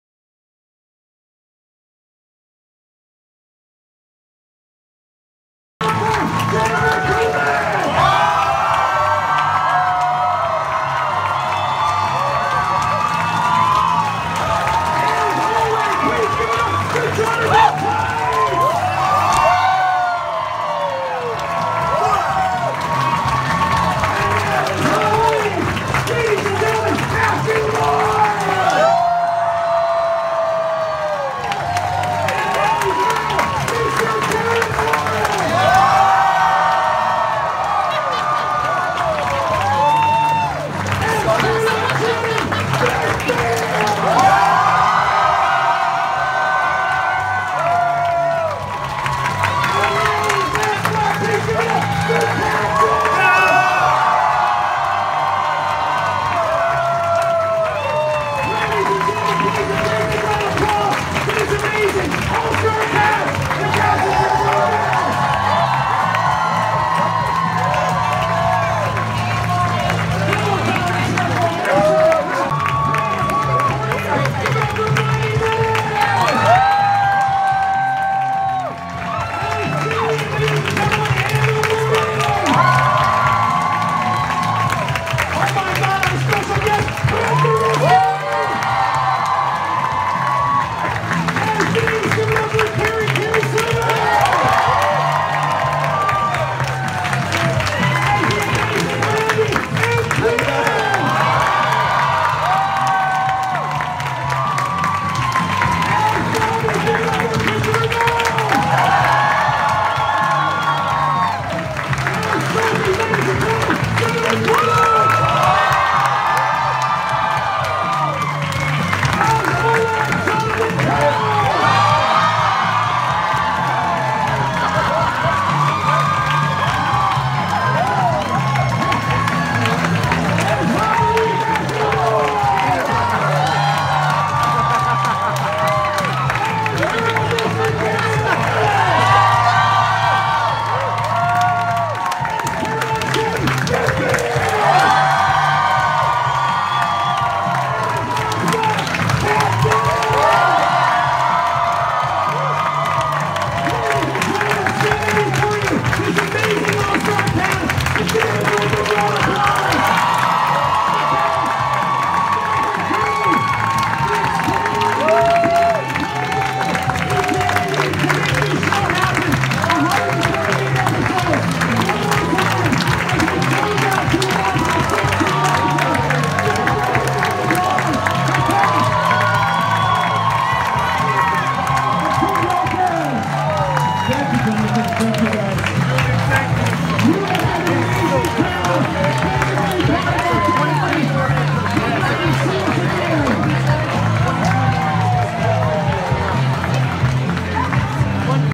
2 Broke Girls Final Curtain Call - March 14, 2017
2 Broke Girls Final Curtain Call - March 14, 2017. This was the last episode of Season 6 and as it turns out the last episode of the series that was cancelled on 5/12/17. The cast includes: Kat Dennings, Beth Behrs, Jonathan Kite, Matthew Moy, Garrett Morris, Jennifer Coolidge, Ed Quinn, Kerri Kenney-Silver, Brad Goreski, and Christopher Gorham. Enjoy